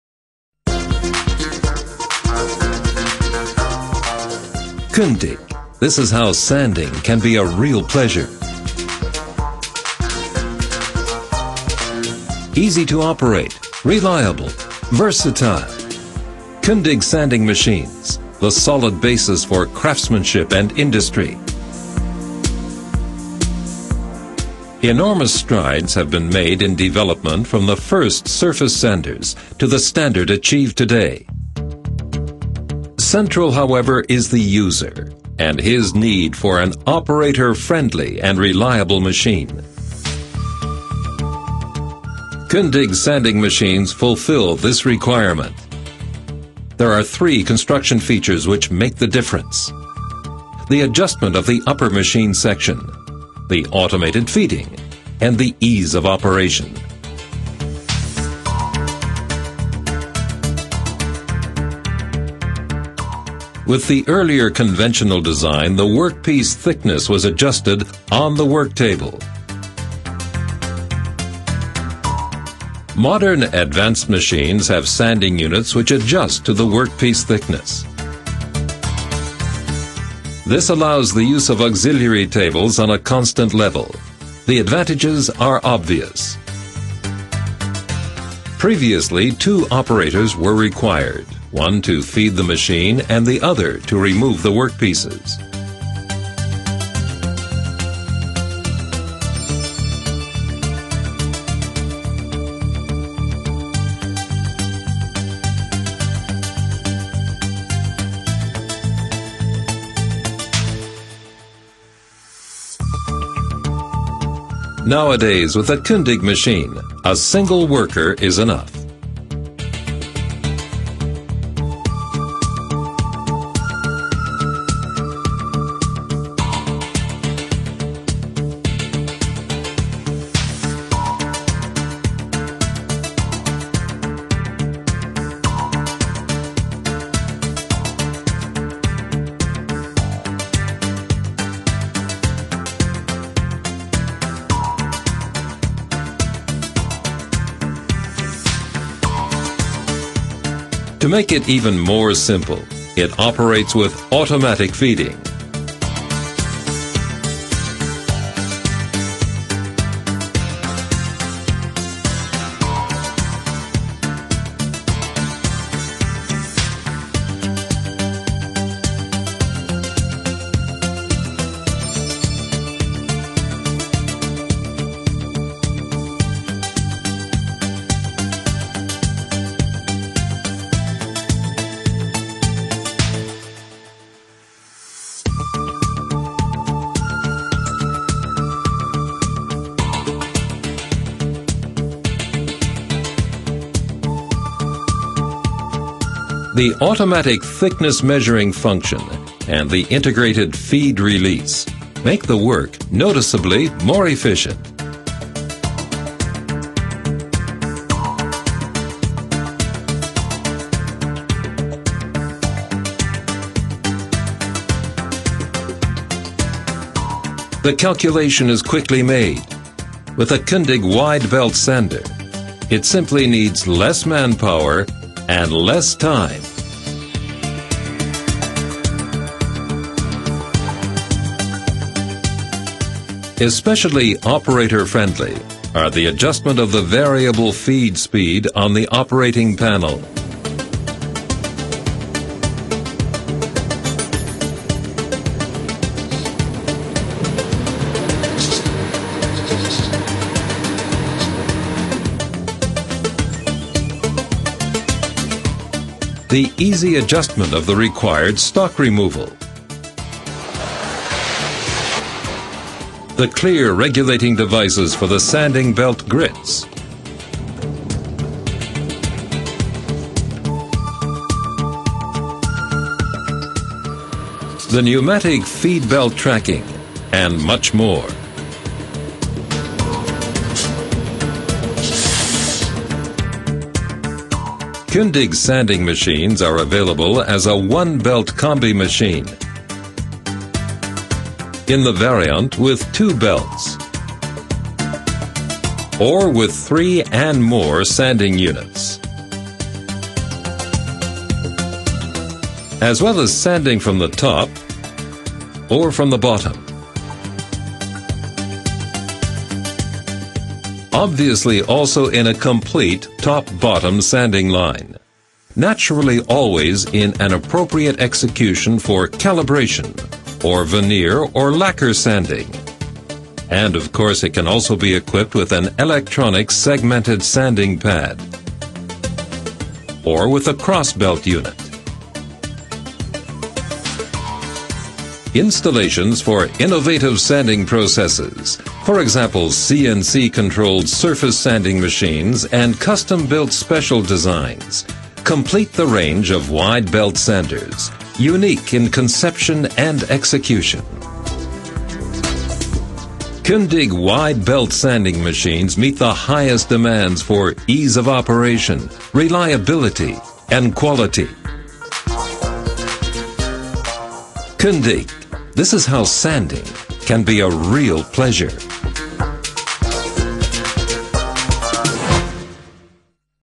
Kündig KUNDIG Premium.flv
Wide range of Kundig wide belt sanders. Planer, calibrating roller, combination pad/roller, Brilliant Oblique Unit, panel cleaners, and top and bottom configurations available. Premium Swiss quality!